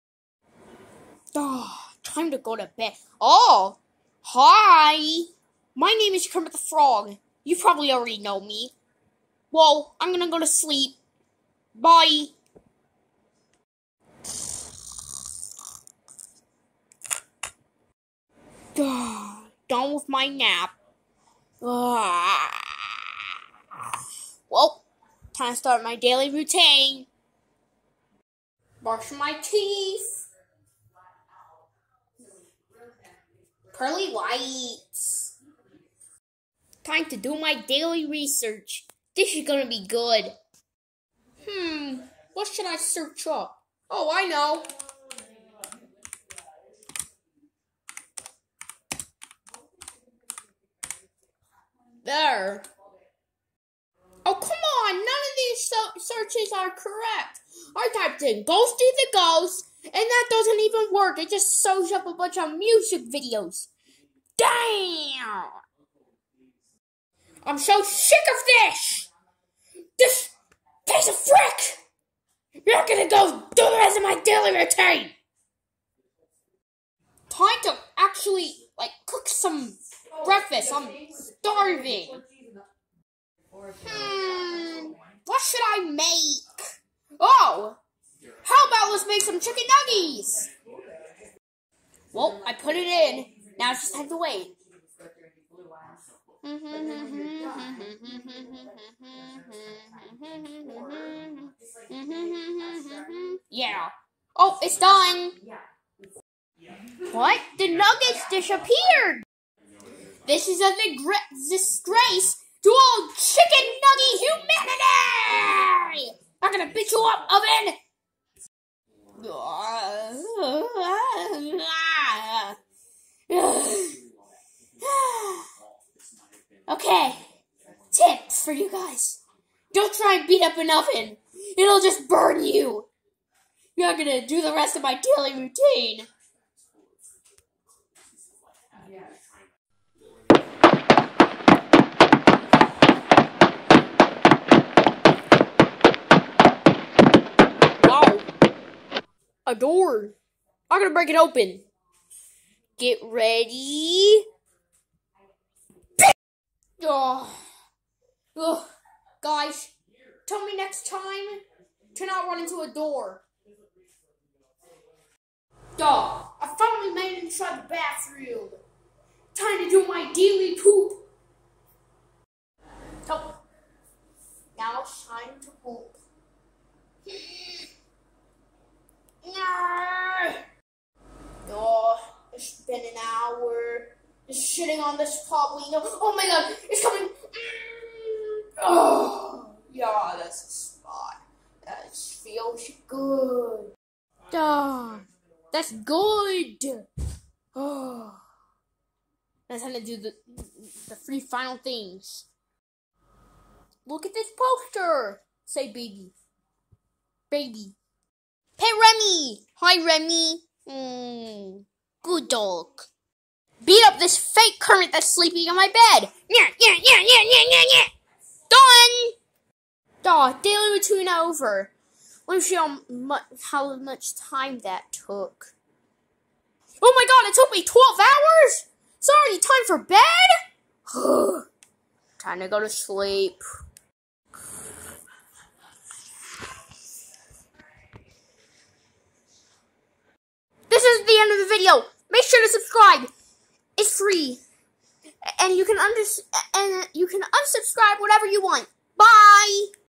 Kermit's daily routine
Kermit has a daily routine...but he is not really happy about it...